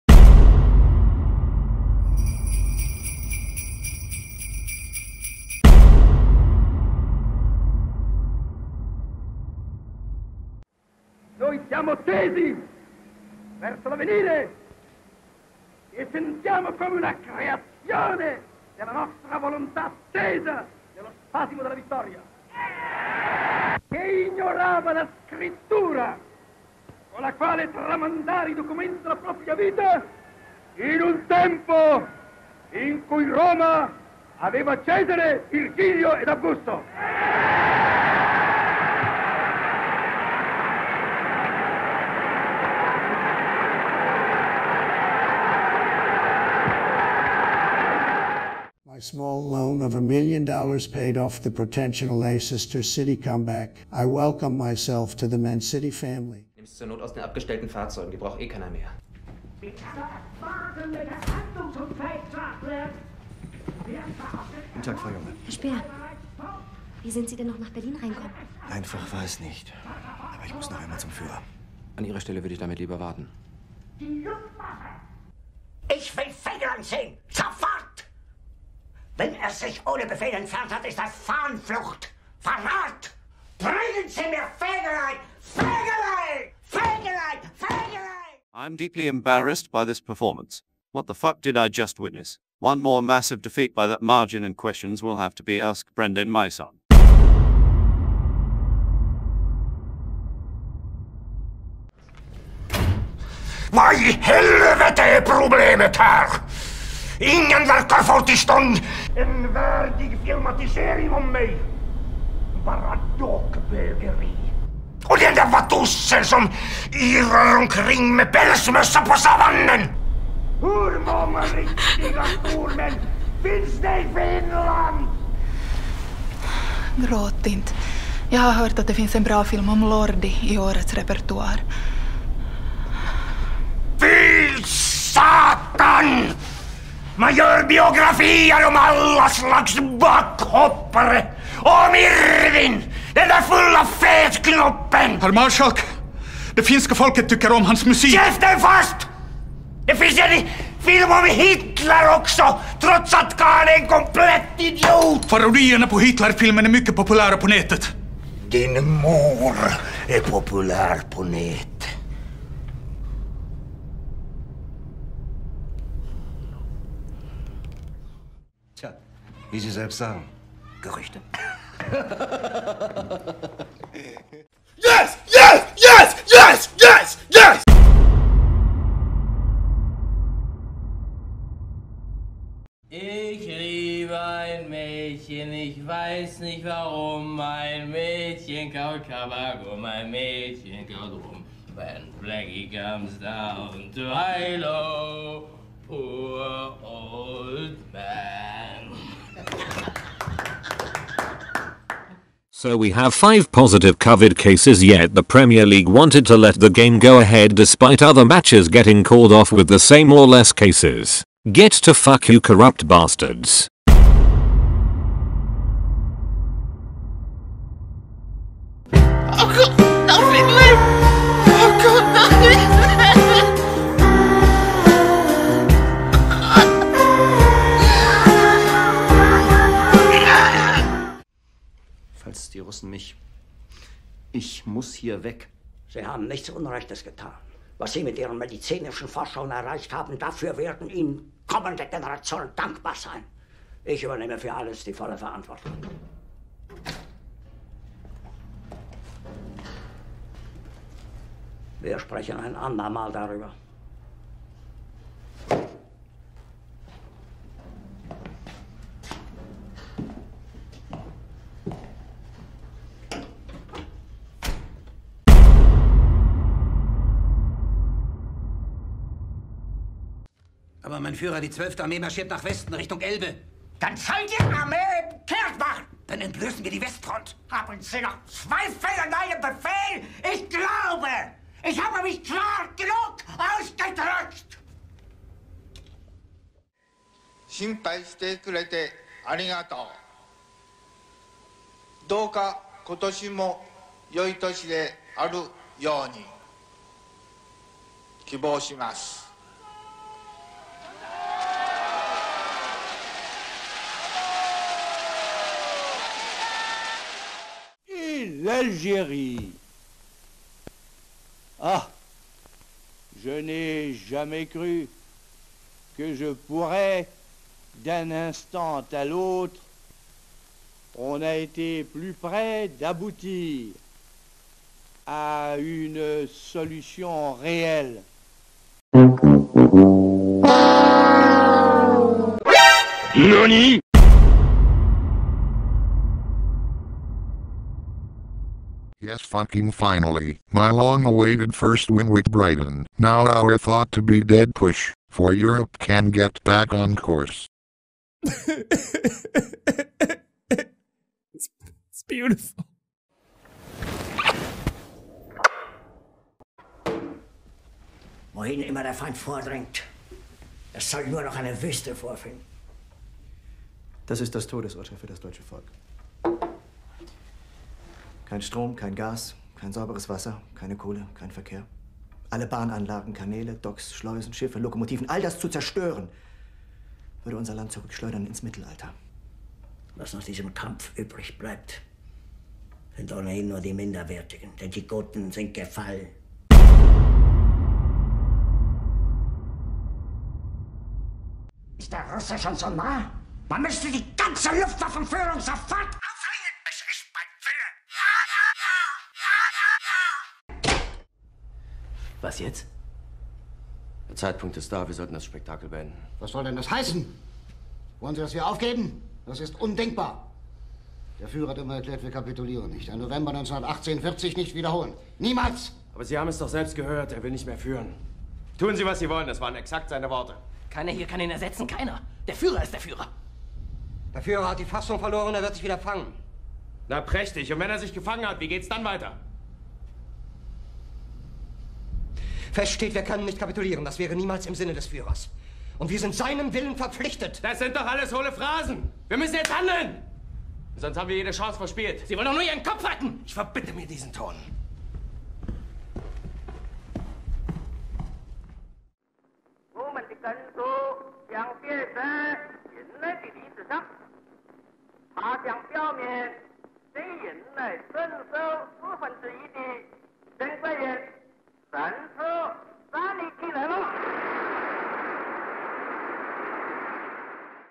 Hitler and the Bunkerians react to the Premier League matchday 19 results
In the premier league fixture the day after Christmas Man City have a new fan and Mussolini side fended off a comeback from Churchill Leicester to win 6-3, Mannerheim first match with Norwich ended in misery being picked apart 5-0 by Krebs and KSIs Arsenal, Burgdorf Spurs beats a depleted Crystal Palace squad 3-0, De Gaulle Aston Villa falls to a 3-1 defeat to Jodl and Hirohito Chelsea and Kollers Newcastle pick a important 1-1 draw against Fegelein and another new fan Manchester United